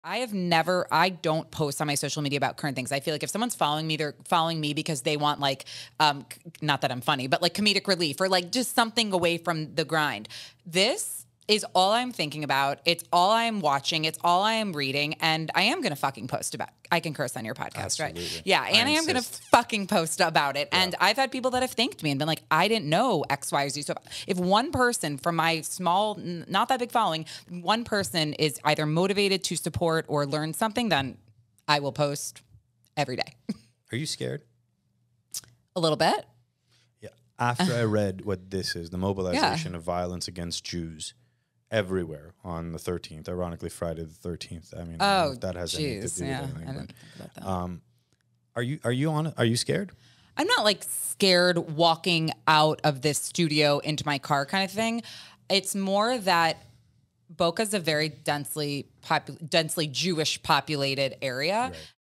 Social Media and Current Events
With everything going on in the world right now, social media is one of the best ways to learn and spread information. Posting important material in hopes that it may teach or inspire others is one of the best things that we can do. Tune in to Episode 39 of Can't Shovel Hurricanes with Julie Lauren to hear more of this important conversation.